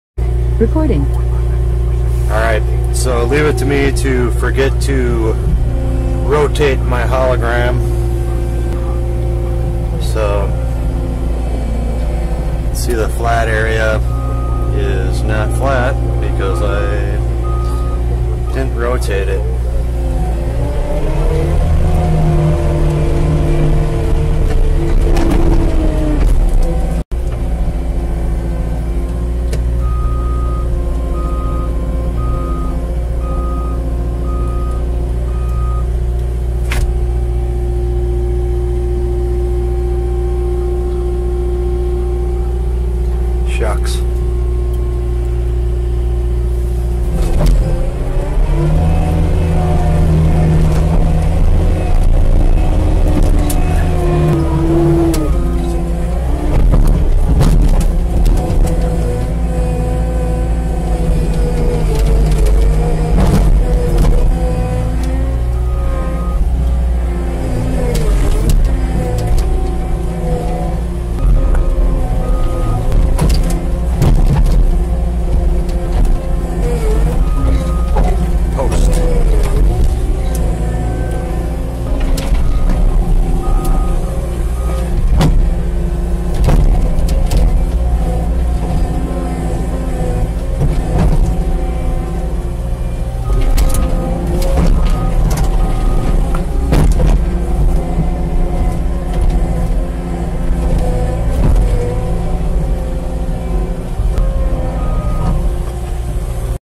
MR Construction Tip: Don't Forget to Rotate Your Hologram!
MR Construction FAIL. Couldn't rotate along x axis on the fly. On the bright side, the plane, material and slope were visible.